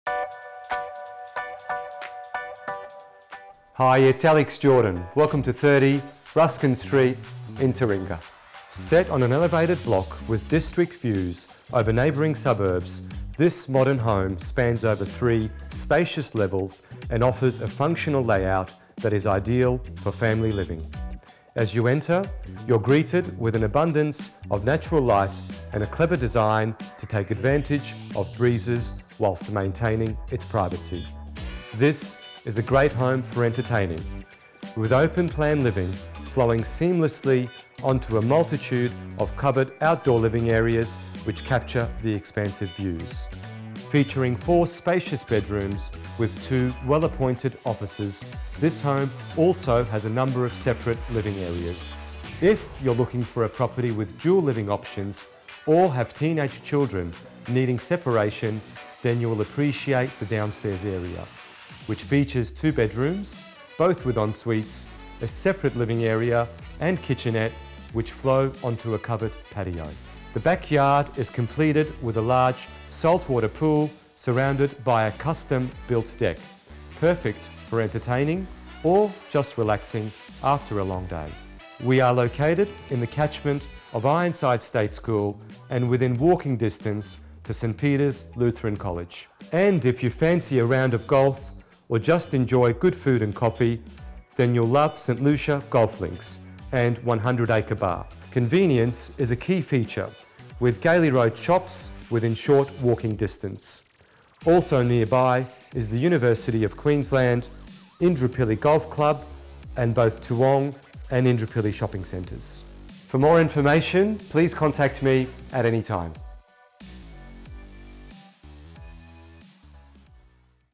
Taringa, 30 Ruskin Street
Set on an elevated block with district views over neighbouring suburbs, this modern home spans over 3 spacious levels and offers a functional layout that is ideal for family living.

As you enter, you’re greeted with an abundance of natural light and a clever design to advantage of breezes whilst maintaining its privacy.

This is a great home for entertaining, with open living spaces flowing seamlessly onto a multitude of spacious. covered outdoor living areas which capture the expansive views.

Featuring 4 spacious bedrooms with 2 well appointed offices, this home also offers a number of seperate living areas - which... makes it the perfect set up for a growing family... looking for space... and convenience.

IF you’re looking for a property with dual living options or have teenage children needing separation then you will appreciate the downstairs area... which features 2 bedrooms, both with ensuites, a separate living area and kitchenette which flow onto a covered patio.

The backyard is completed with a large salt water pool surrounded by a custom built deck - perfect for entertaining guests or just relaxing after a long day.

Whilst this home is beautifully presented throughout - it is its position that makes it really special.

We are located in the catchment of Ironside State School and within walking distance to St Peters Lutheran College. And if you fancy a round of golf or just enjoy good food and coffee... then you’ll love St Lucia Golf Links and one hundred acre bar - less than 5 minutes walk from here.

Convenience is a key feature with Gailey road shops also within short walking distance.

Also nearby is the University of Qld, indooroopilly golf club and both Toowong and Indooroopilly shopping centres.